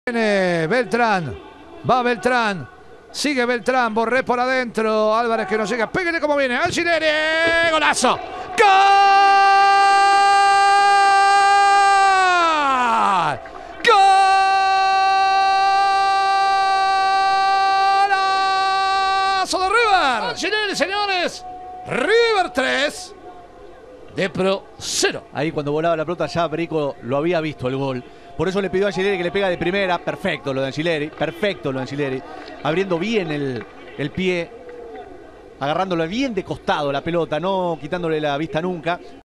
GolazosCopaArgentina: Fabrizio Angileri
Mirá el tanto del defensor en la victoria de River por 4-0 ante Defensores de Pronunciamiento, por los 32avos de Final de la edición 2020.